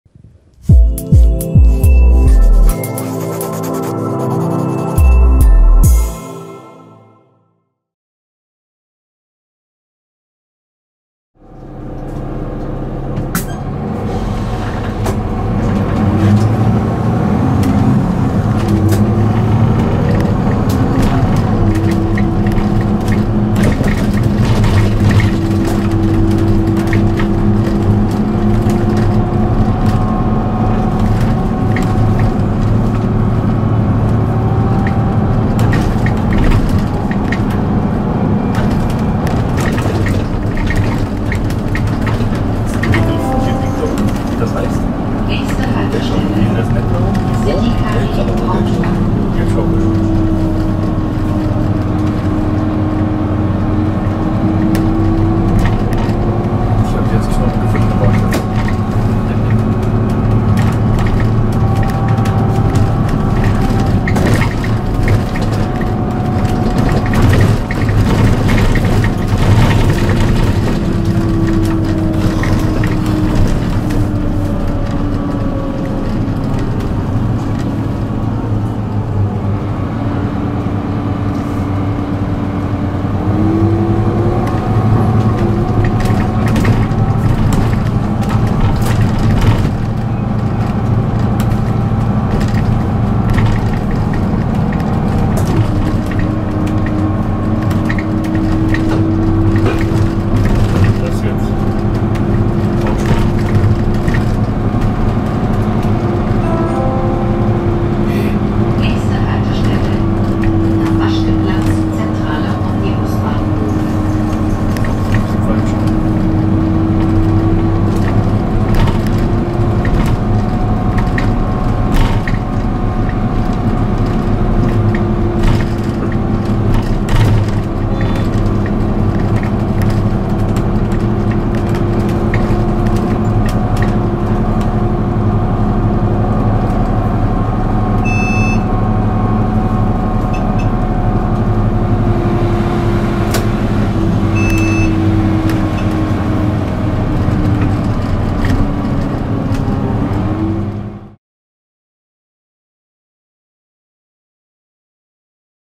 Magdeburg - Bahnhofsbrücken - vom Allee Center bis zum Damaschkeplatz
Mit einem Solaris Urbino 12 geht es 2015 zum Nachtanschluss von Cracau zum Damaschkaplatz und zurück. Dort trafen alle nächtlichen  Straßenbahn- und Buslinien den Magdeburger Verkehrsbetriebe (MVB) zu einem zentralen Umsteigepunkt zusammen. Dabei werden die alten Bahnhofsbrücken unterquert. Teile der Baustelle zum Citytunnel sind bereits sichtbar.